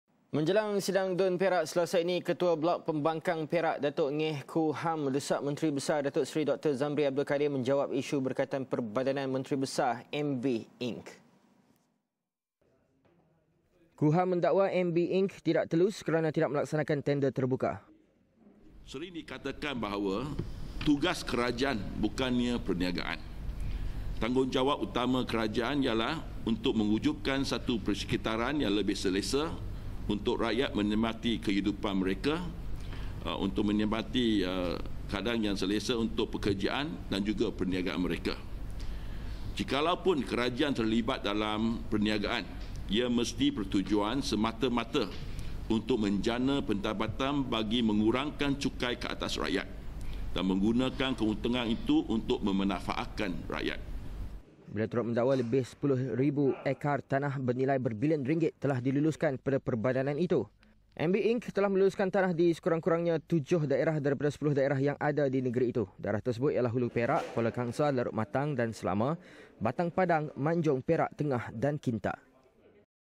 MB Inc luluskan tanah projek berbilion ringgit- Ngeh Koo Ham
Menjelang sidang DUN Perak Selasa ini.

Ketua blok pembangkang Perak, Datuk Ngeh Koo Ham mendesak Menteri Besar Datuk Seri Dr Zambry Abdul Kadir menjawab isu berkaitan perbadanan Menteri Besar.MB Inc.